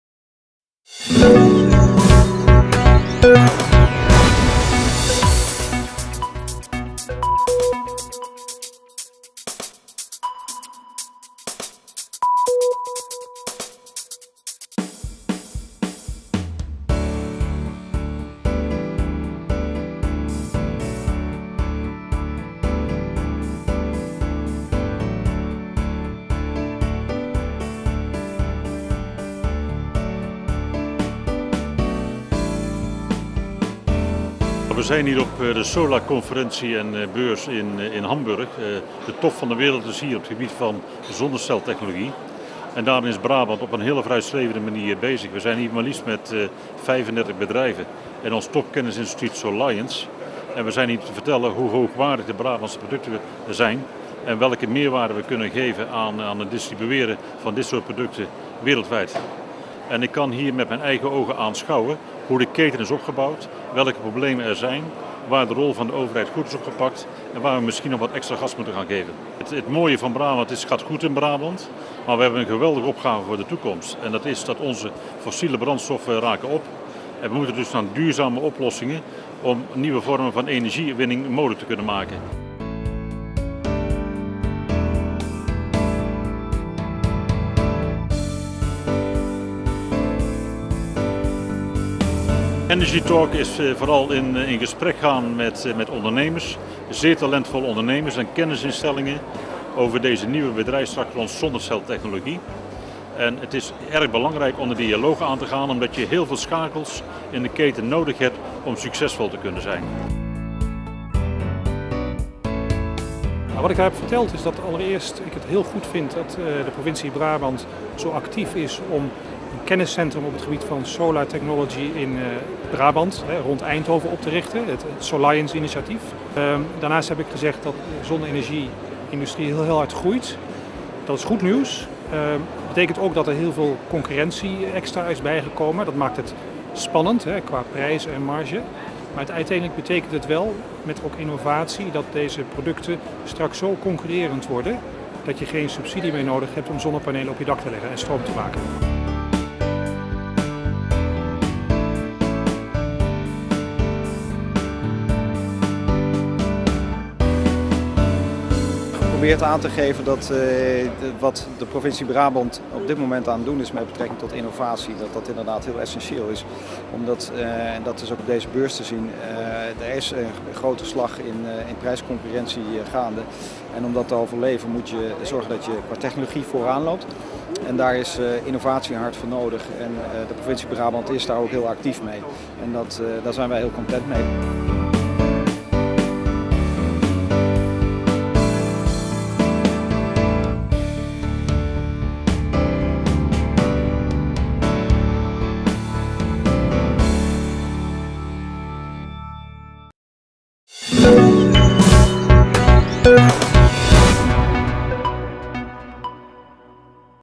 Brabant op Solar-beurs
Brabant was prominent aanwezig op de grootste solar-beurs (European Photovoltaic Solar Energy Conference and Exhibition) die begin september 2011 in Hamburg plaatsvond. Bedrijven, kennisinstellingen en de provincie trokken samen op.